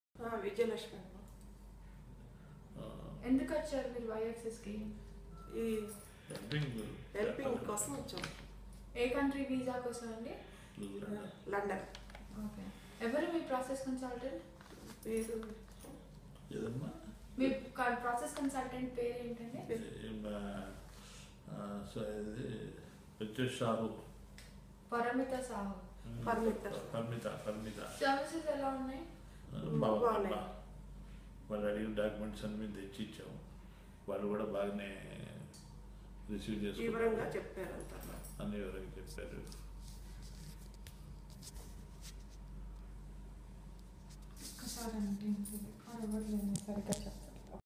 Y-Axis Feedback|K Bhaskar Rao Review On His UK Family Visit Visa Processing.
K Bhaskar Rao Gives us Y-Axis Review for a country Family visit visa As he satisfied with the services provided by Y-Axis Concern Team to reach the destination Safely.